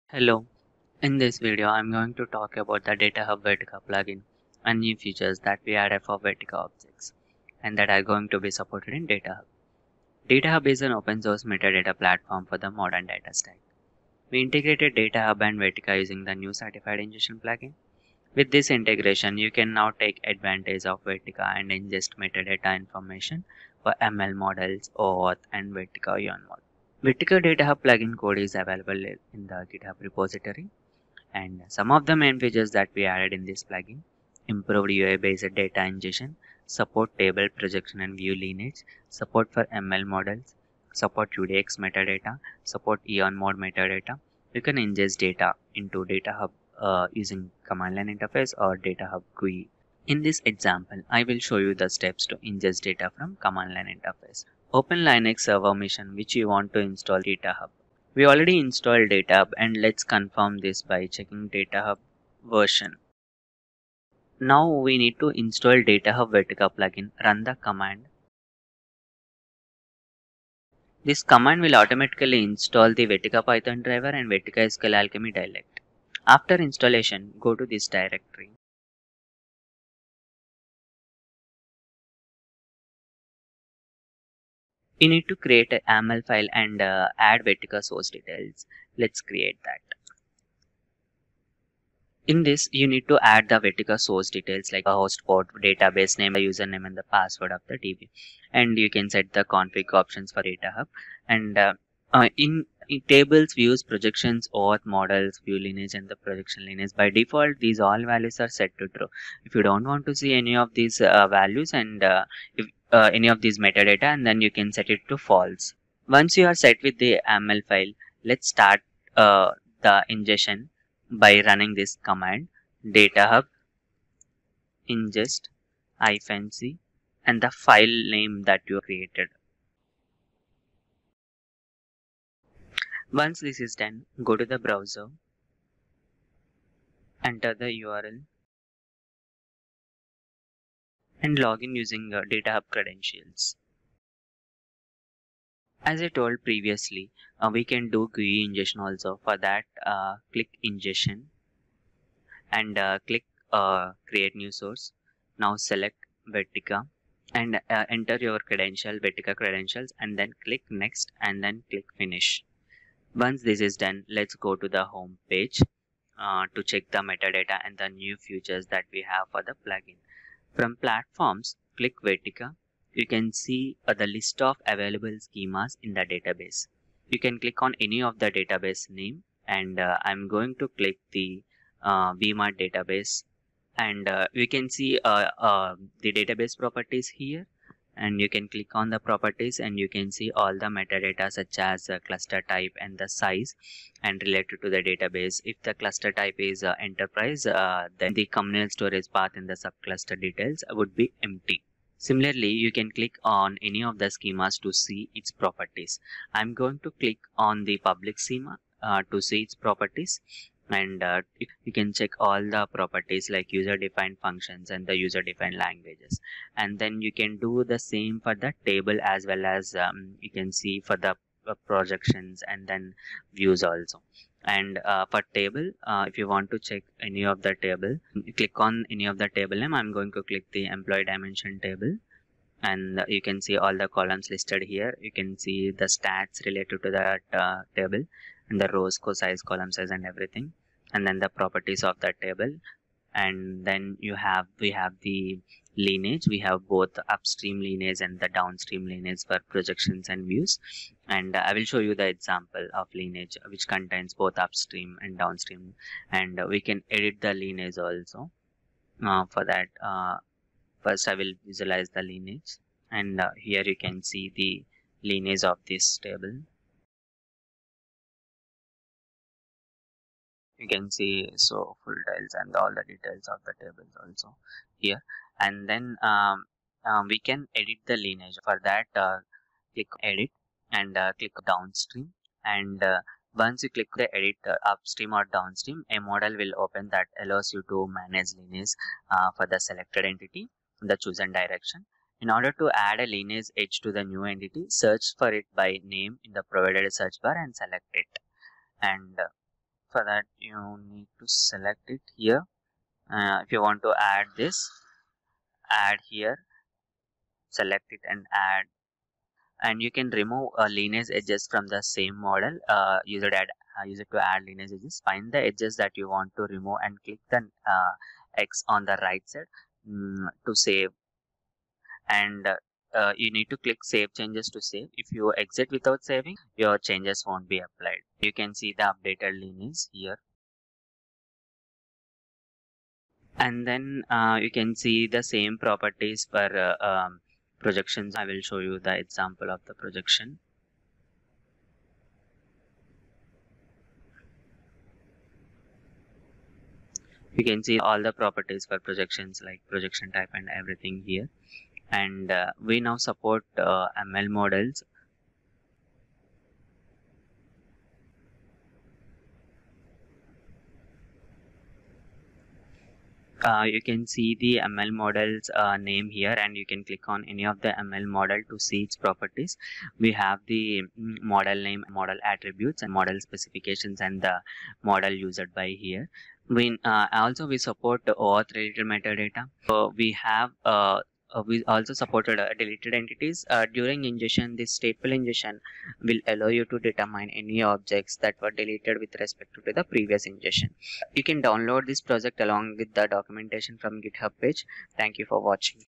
Ingesting Vertica Metadata in DataHub
DataHub is a metadata platform for end-to-end lineage and data discovery. This video demonstrates how to effectively incorporate Vertica data in DataHub using the DataHub Vertica Plugin, enabling you to leverage the platform's advanced capabilities for gaining deeper insights.